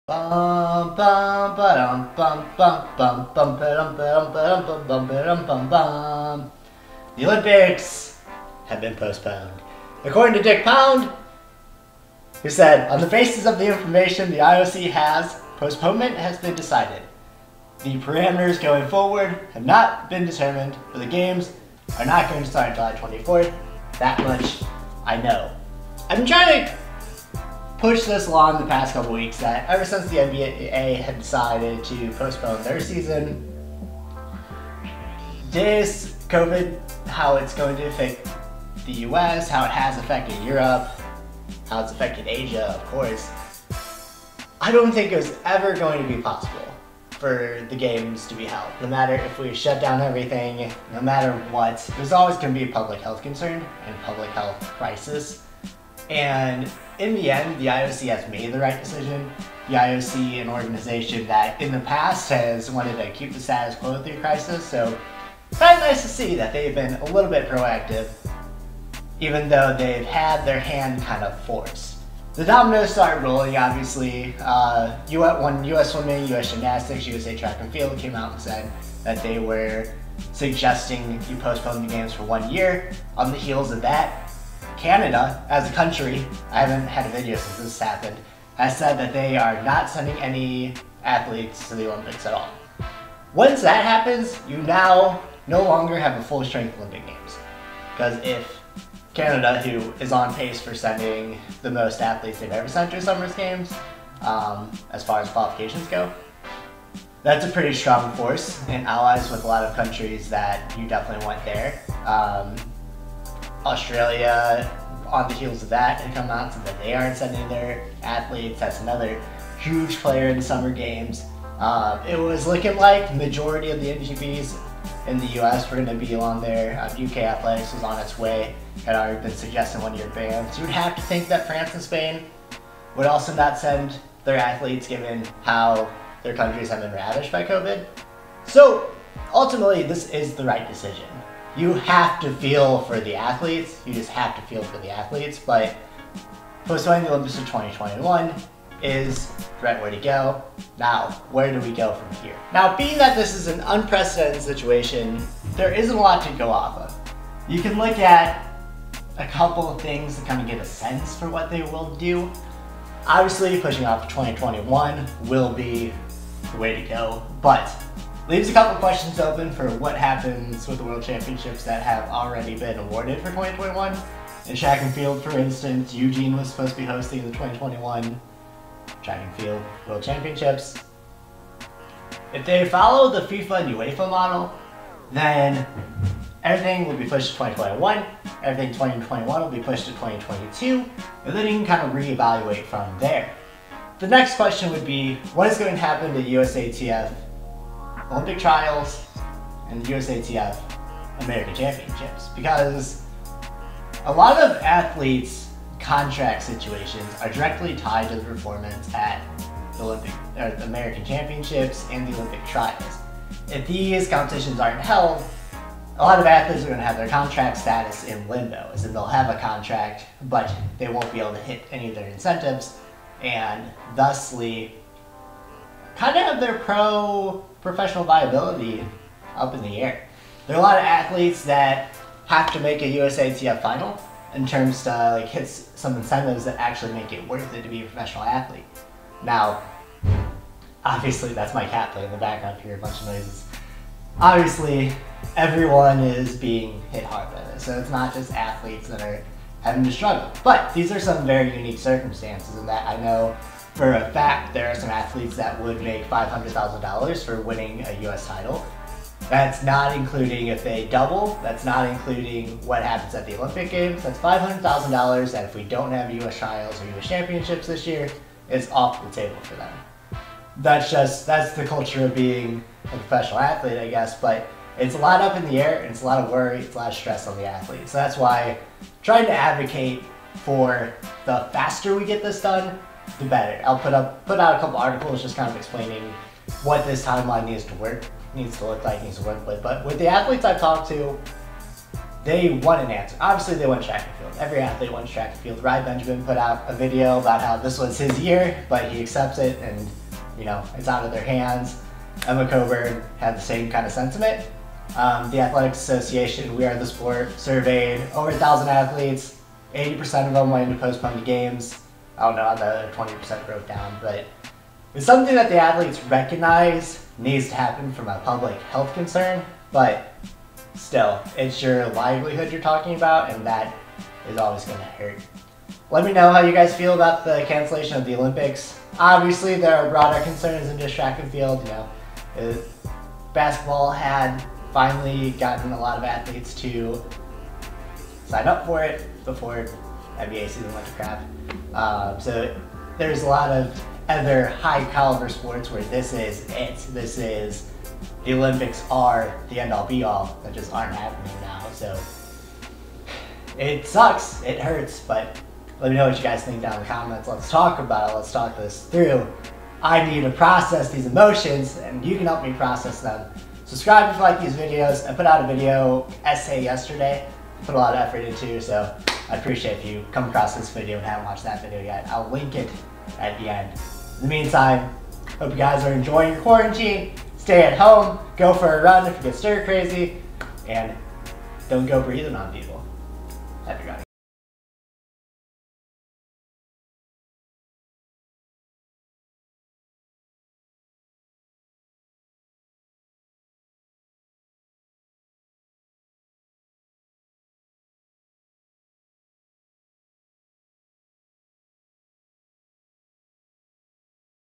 The Olympics Are Postponed... Hopefully Not Cancelled
Dick Pound made the announcement today that the Olympics will be postponed and held off until 2021. Here are my initial thoughts on Tokyo2020 becoming